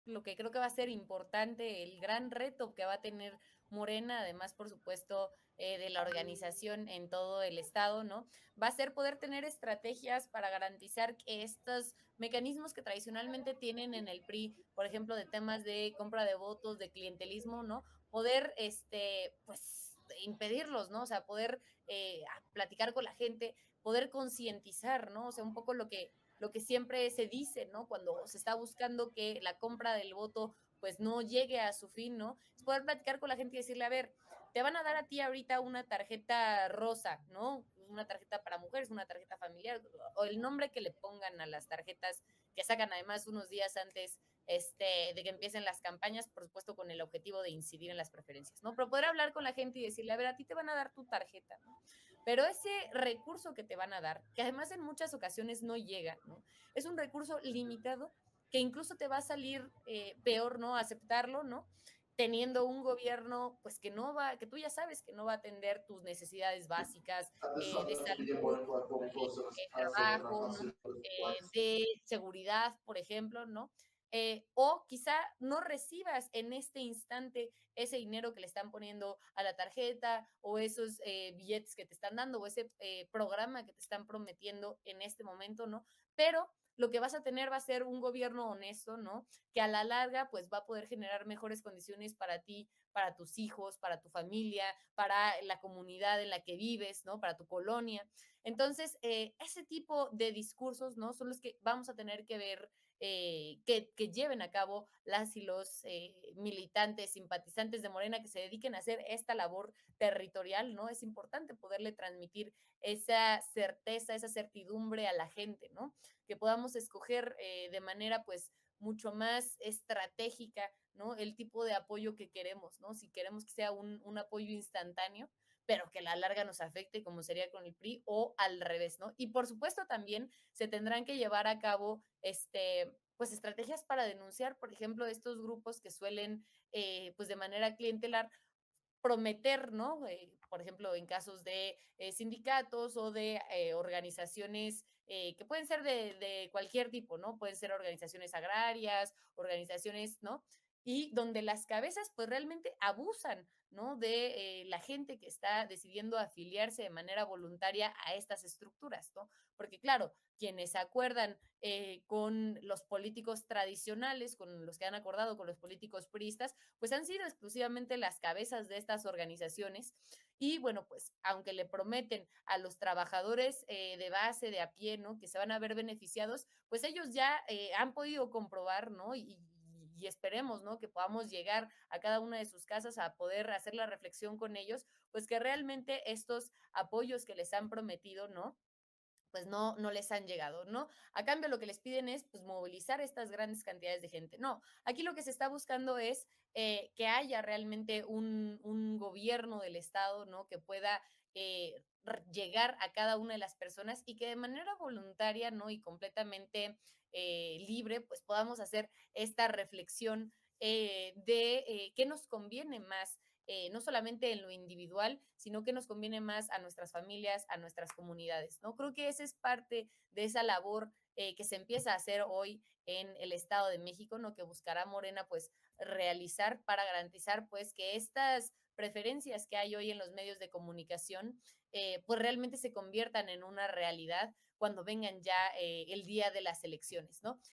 MORENA ANTE EL RETO DE FRENAR USO POLÍTICO DE LA TARJETA ROSA EN EDOMEX
Es Tiempo de Morras la comunicóloga política Camila Martínez y la periodista y reportera de la revista Polemon, Beth Castillo, hablan del reto de Morena en el arranque de las campañas para renovar la gubernatura del Estado de México.